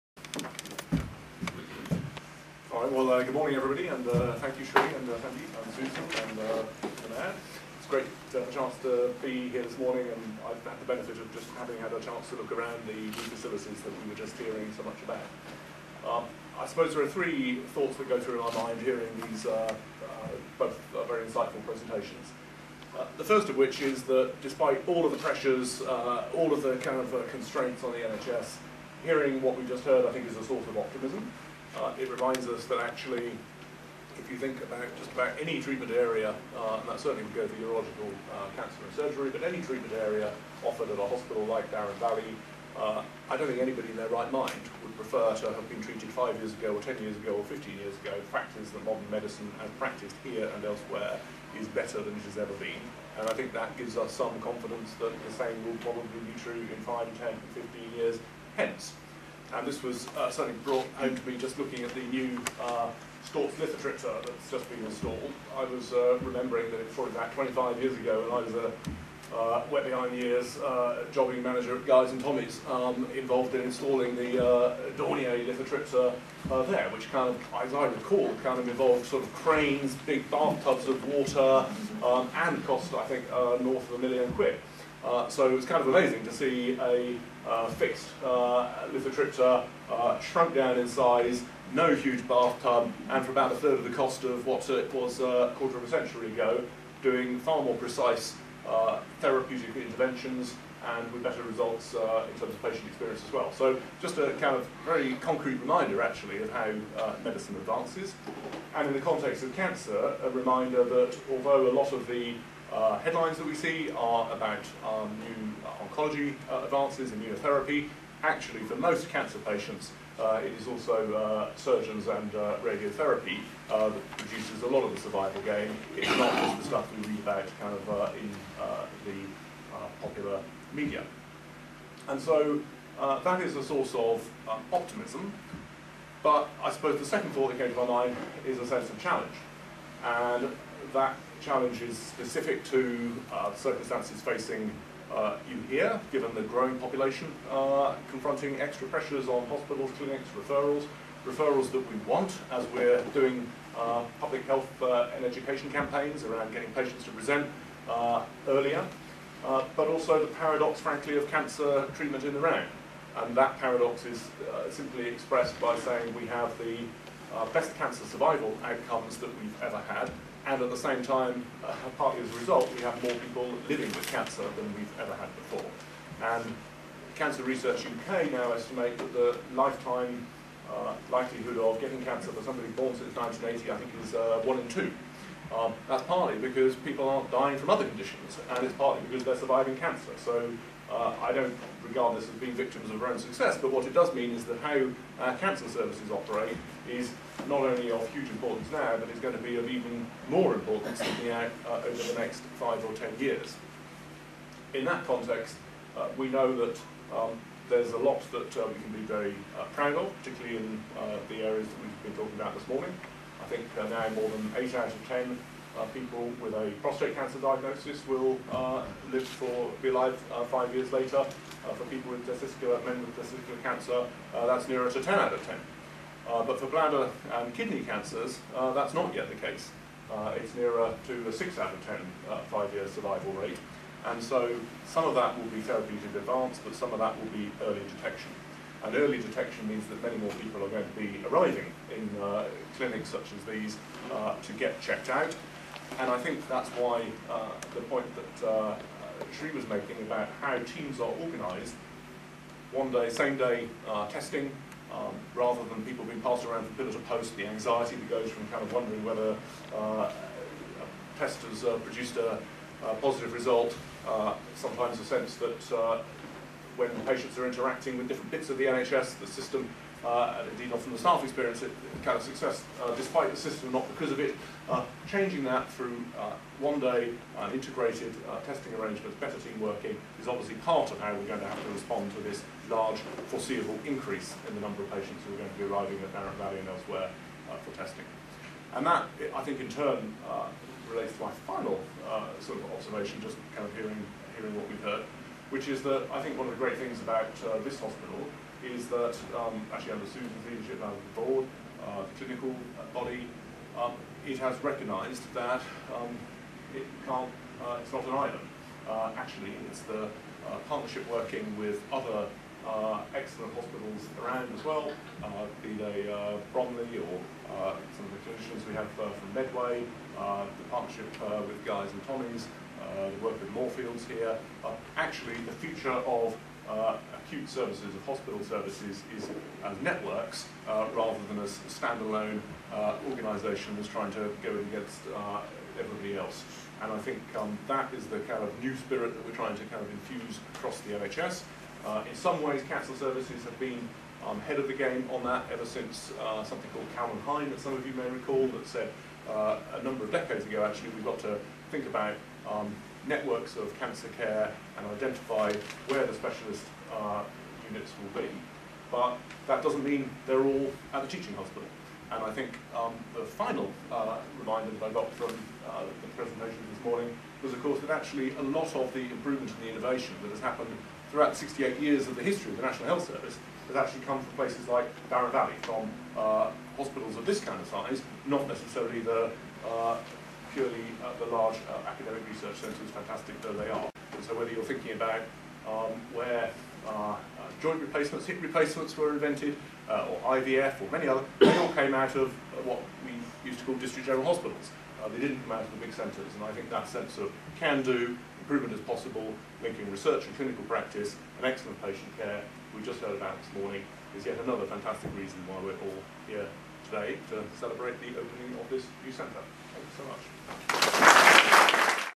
Simon Stevens Opens One-Stop Urological Cancer Diagnostic Centre at Darent Valley Hospital
Chief Executive of NHS England, Simon Stevens, formally opened a new One Stop Urological Cancer Diagnostic Centre at Darent Valley Hospital, on 14th October 2016.

The Urology Team have established a one-stop Urological Cancer Diagnostic Centre based in the Poplar Unit. It hosts a full range of outpatient cancer services including a one-stop Haematuria Clinic (patients presenting with blood in the urine are fully assessed and investigated, giving them the diagnosis in one visit) and Prostate Biopsy Clinic (for patients with suspicion of prostate cancer).

During his visit Mr Stevens reviewed the urology facilities at the hospital and saw first-hand how we remain at the forefront of urological treatment in the region. The opening of the One Stop Urological Cancer Diagnostic Centre at Darent Valley Hospital represents the next level in the management of urological cancers for local people. The new unit delivers far more comprehensive treatment and care in one visit. The one stop service enables patients to have all the necessary tests and diagnosis of their condition during the same visit. Waiting for the results of a test for cancer can be extremely stressful and this new service helps relieve anxiety and offers reassurance.

This centre of excellence also provides all forms of treatment for kidney stones including lasers, key-hole surgery and many other procedures. Several thousand people are seen at the hospital each year suffering from urological cancers and stone disease.

A new state-of-the-art fixed site, dual localisation (x-ray and ultrasound) Stortz Lithotripter also brings the treatment of kidney stones into the new era.  Used to remove kidney stones without surgery by shattering stones with a series of pulses, the new machine can treat both visible and non-visible stones.  X-rays are used to spot and target the stones and now ultrasound is used to show up stones that previously went undetected by x-rays.

The centre also has metabolic stone unit to help prevent reoccurrence of stones, offering appropriate medical and dietary advice. The centre is also active in basic and clinical stone research with many research fellows.

Director of the Centre, Professor Sriprasad, says “With all of these facilities we can now offer a comprehensive service to our local community that is the best in the region. The opening of the new centre means that our patients get the most appropriate treatment, faster and closer to home than ever before. The facilities that we provide are comparable with the best in the country.”

Professor Sanjeev Madaan, Lead Cancer Surgeon for Dartford and Gravesham NHS Trust added: “The urology team at Darent Valley currently offers a world class service for the diagnosis and management of urology cancer patients and I feel proud to be part of this team.”